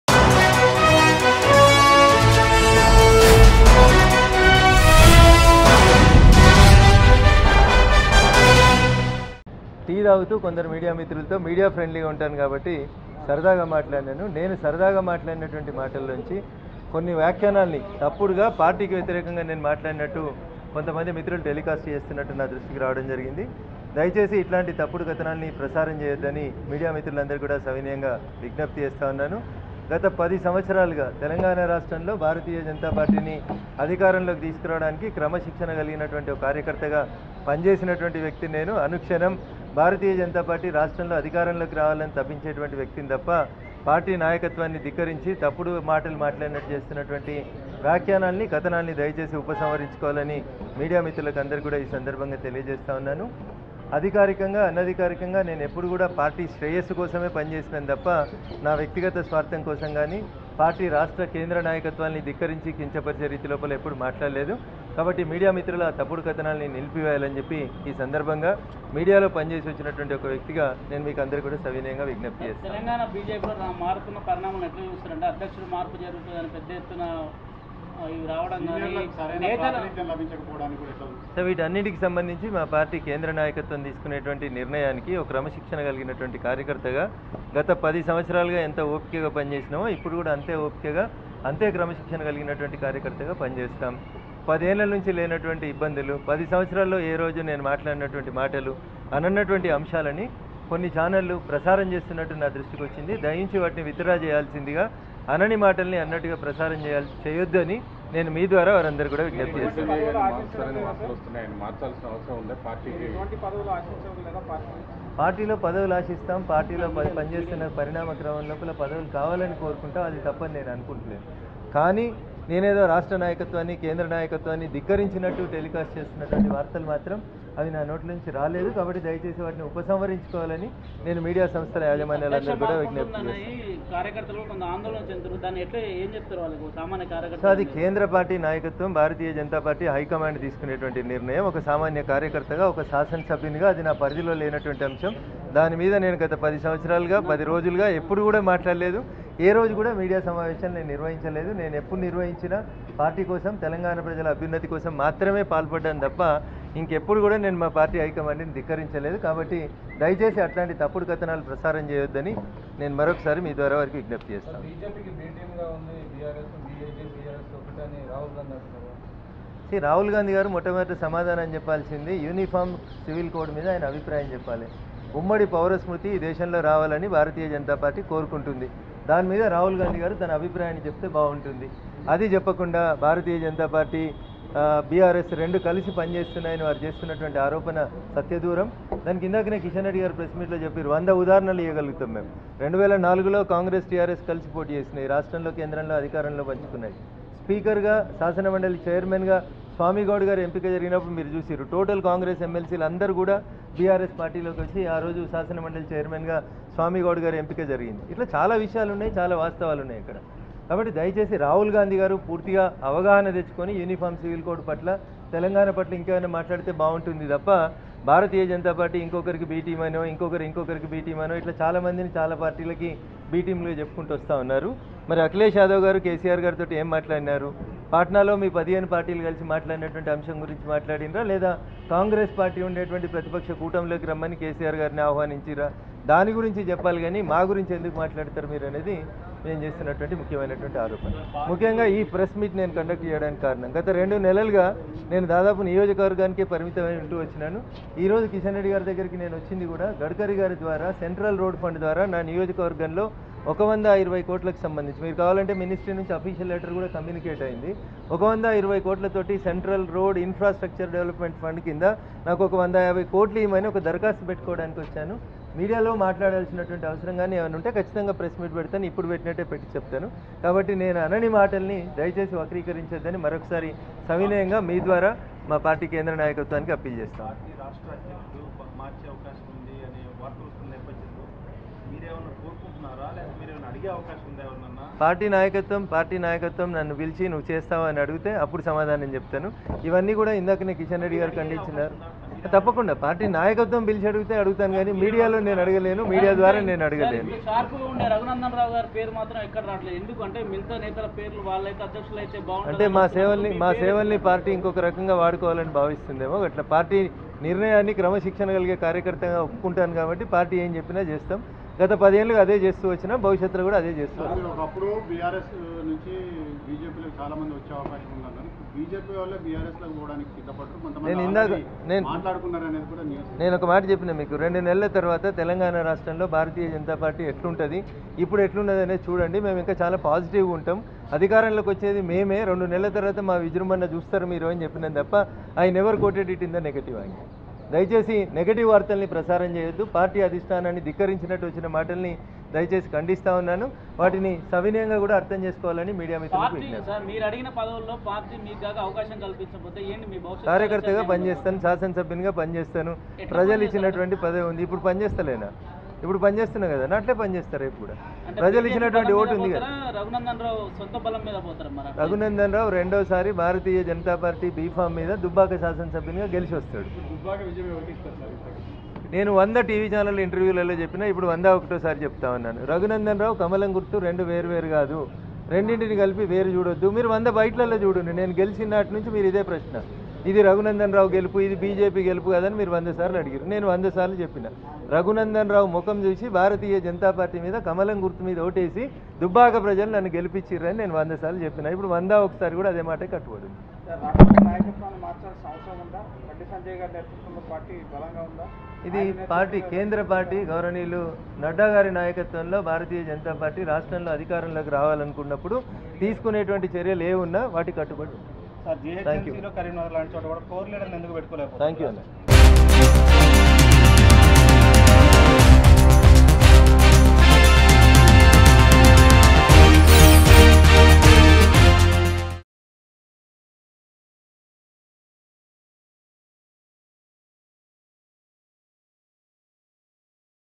BJP Dubbaka MLA Raghunandan Rao Opens Up About Party Change | Telangana Political News | Mango News
BJP Dubbaka MLA Raghunandan Rao Opens Up About Party Change.